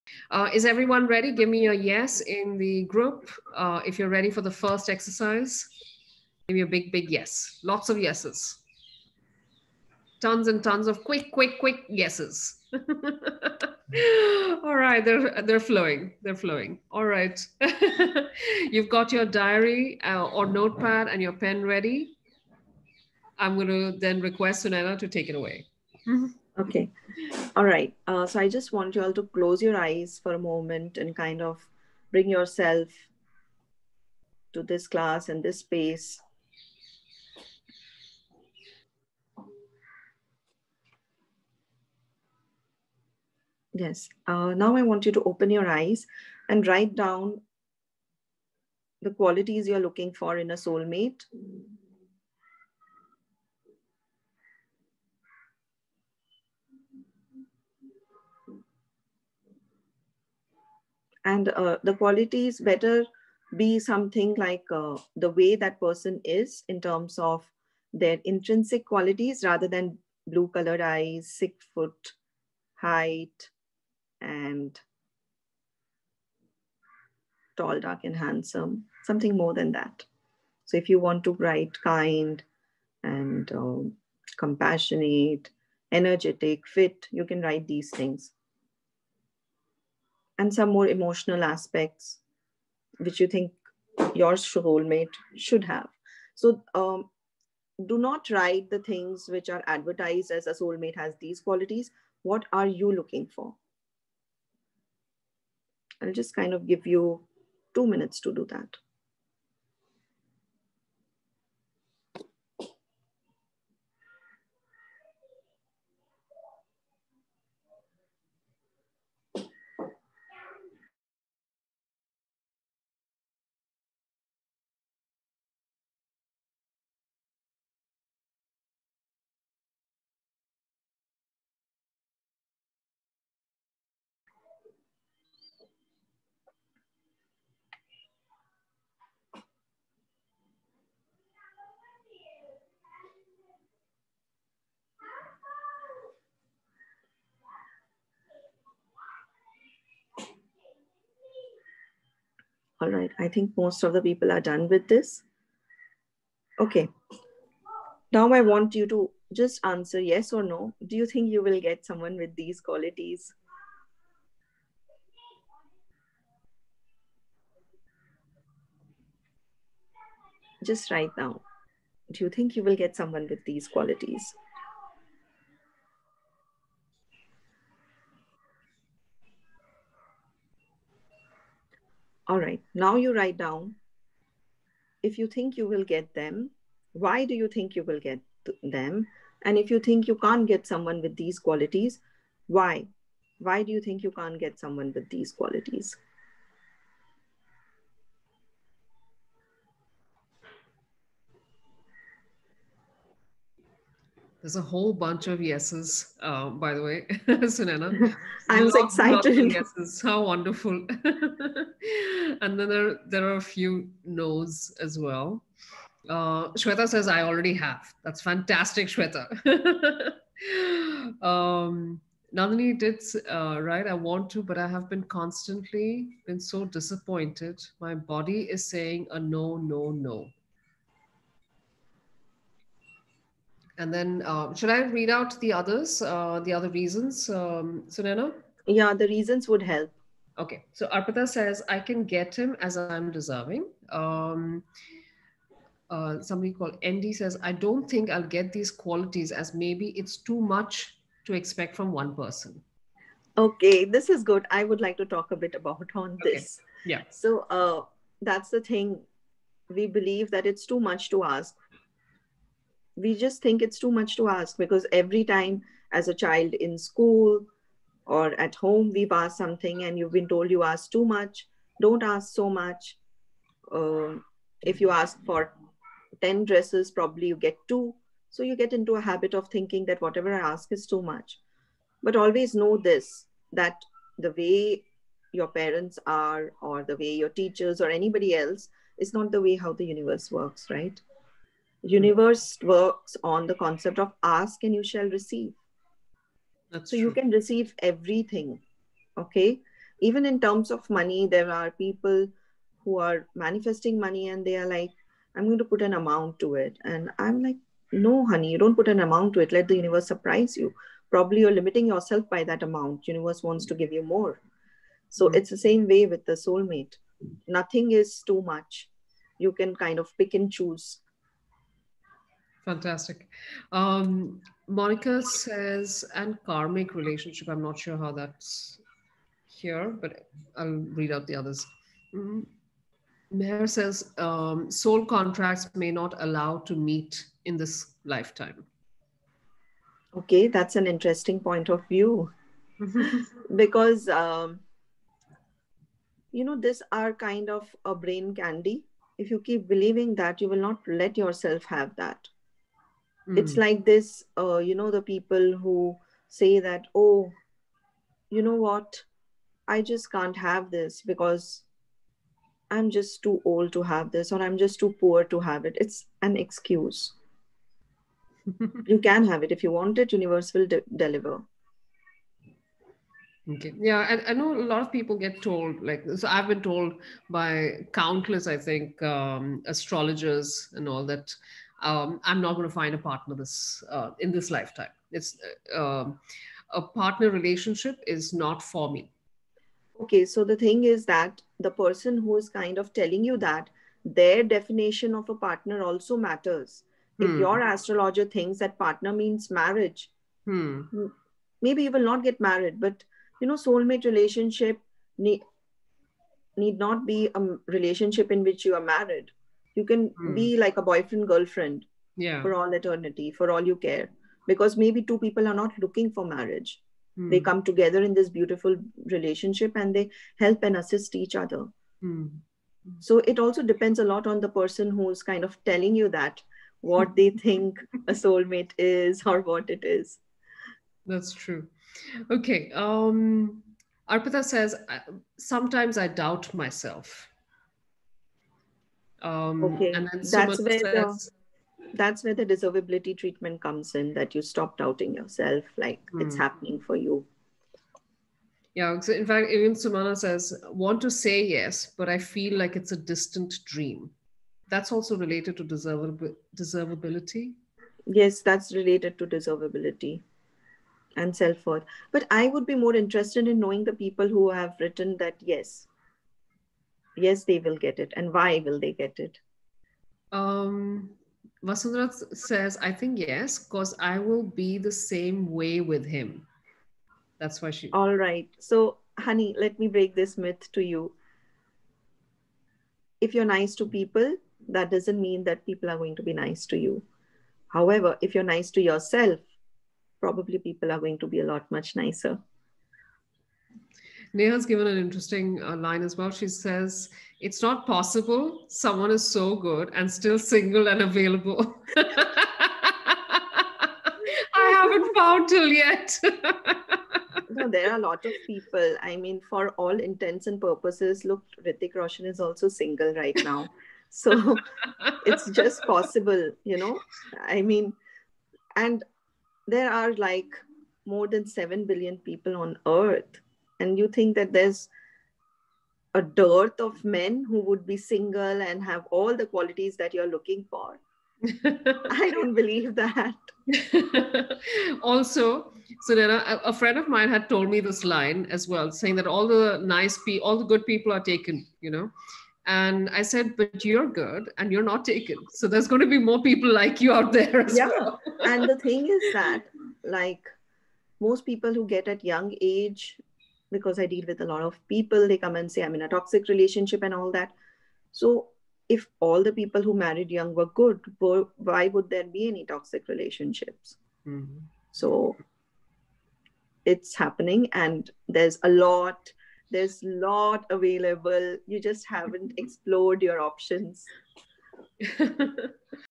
Sunainaa shares a simple exercise to Visualise & attract your soulmate -The Mystic Lotus Masterclass
Sunainaa Agrawwal answers questions about the difference between soulmates, twin flames, life partners and breaks down some complex relationship issues with easy explanation and simple solutions on The Mystic Lotus and Dragonflies Soulmate Alignment Masterclass.